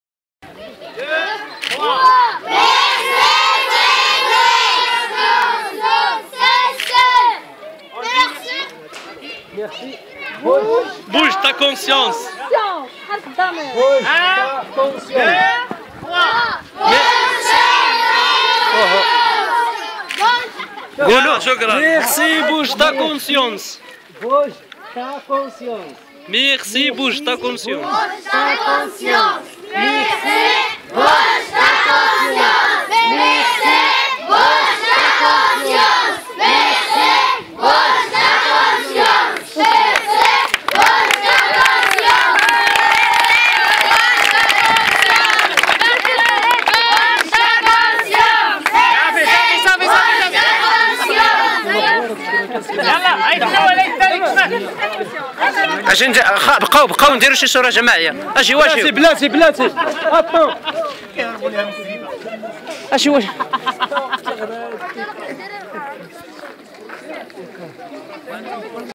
Circuit Humanitaire (11 décembre 2014): les enfants d'Imintidgt
Lors de notre circuit humanitaire au travers des villages des montagnes, après chacune de nos distributions de cartables aux enfants; nous prenons la peine de leur expliquer qui nous sommes et d'ou nous venons.
Nous prenons le temps d'expliquer que de merveilleux donateurs qui, pour beaucoup, n'ont jamais été au Maroc, ont souhaités faire un geste pour les aider.
C'est pourquoi ces enfants en remerciant BTC et asbl avec qui nous collaborons maintenant depuis 3 ans, remercient en réalité chacun d'entre vous.

Ici: tous les enfants de l'école primaire d'Imintidgt (1ere étape de notre circuit humanitaire)